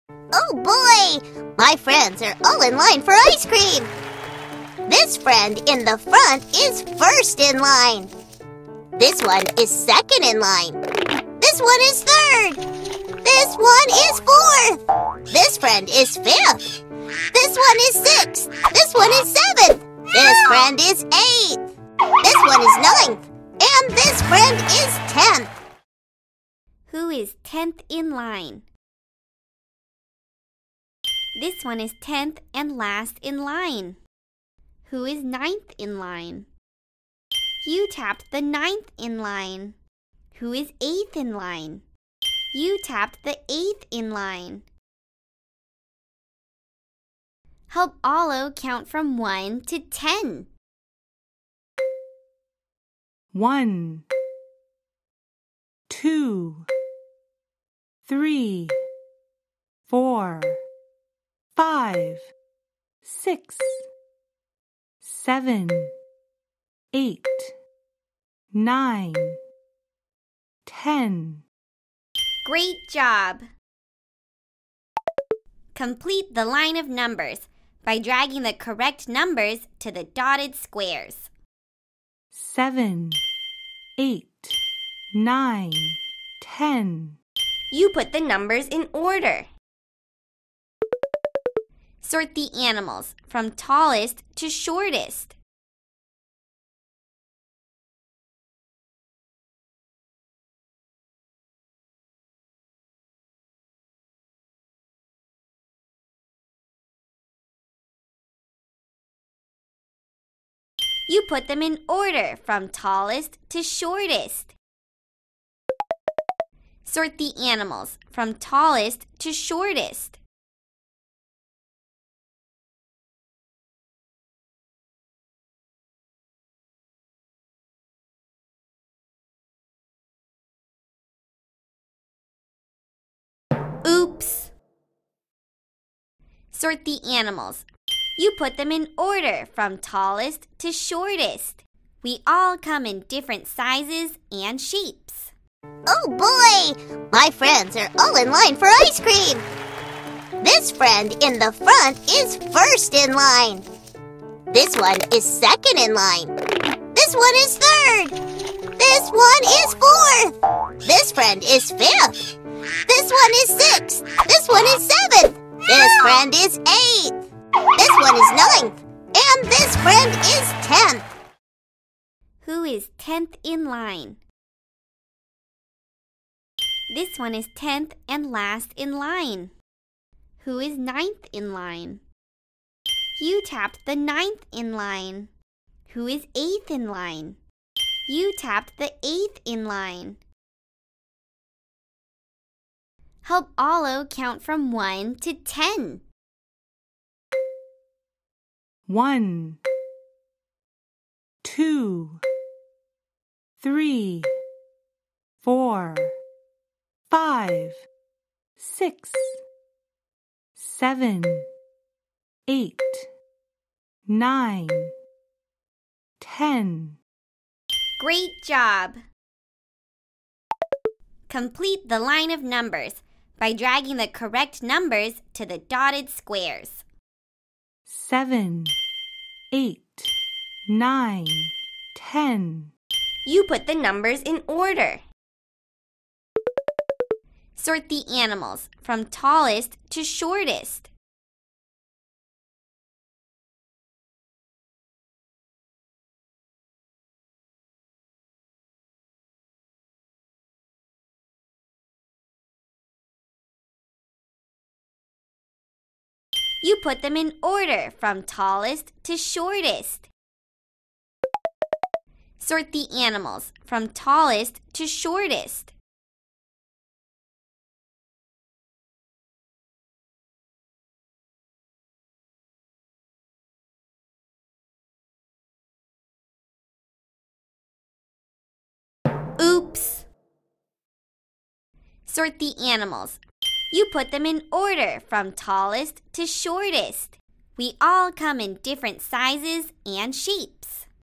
Seventh, Eighth, Ninth, Tenth - First in line - Counting numbers for kids
Learn numbers counting by first in line for children and kids.